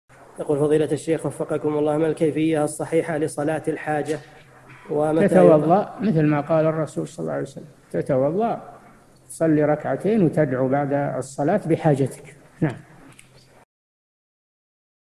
[1007 /1350] ما الكيفية الصحيحة لصلاة الحاجة؟ الشيخ صالح الفوزان
-[1007 /1350] السؤال: ما الكيفية الصحيحة لصلاة الحاجة؟ وأين مكان الدعاء الذي يقرأ فيها؟
الجواب: تتوضأ مثلما قَالَ الرسول صلى الله عليه وسلم ، تتوضأ، تصلي ركعتين وتدعو بعد الصلاة بحاجتك.
للاشتراك في قناة الشيخ صالح الفوزان
أسئلة وأجوبة من شرح قاعدة جليلة ( 1350 ) مقطع

للاستفسار والإقتراحات والدعم
رئيس الفريق
لمشروع كبار العلماء